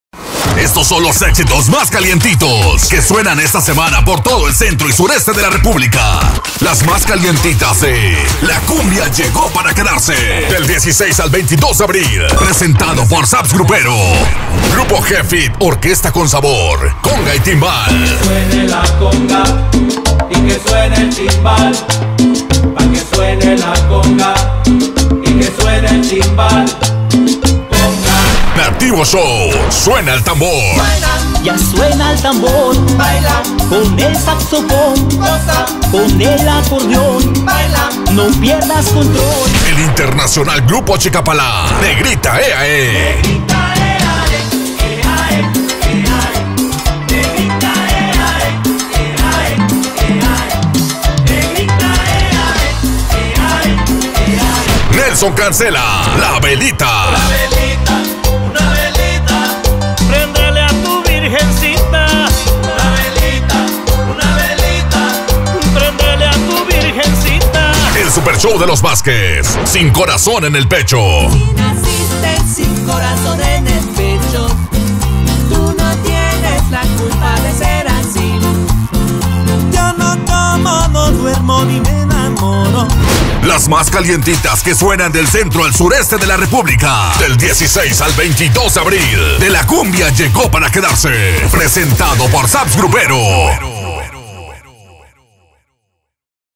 Las Más Calientitas de la Cumbia del 16 al 22 de abril
Estas son las canciones más calientitas de la cumbia, que suenan en el Centro y Sureste del país. Solicítalas en las radios que transmiten La Cumbia Llegó Para Quedarse.

¡La Cumbia Llegó Para Quedarse!